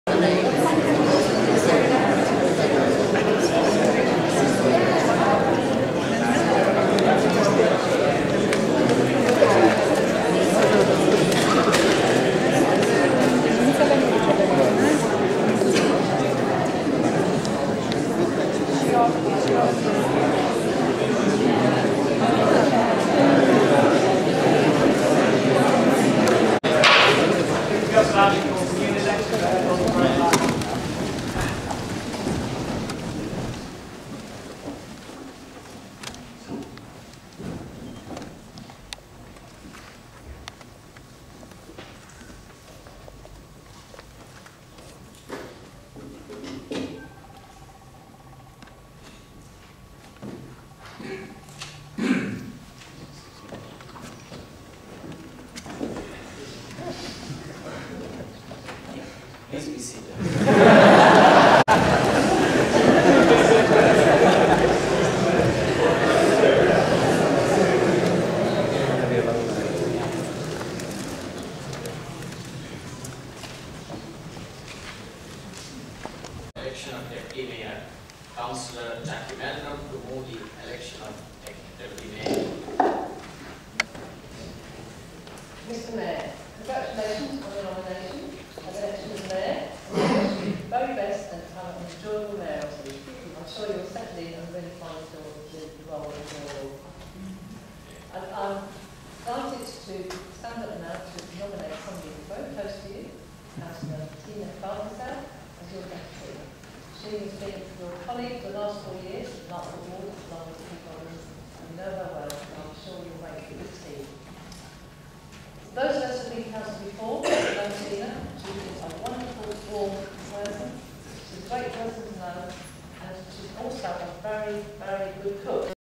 Mayor Dr Neeraj Patil.VOB.007
Mayor Councillor Dr Neeraj Patil Taking Oath as a Mayor Of Lambeth Council,London on 26 th May 2010 at 7pm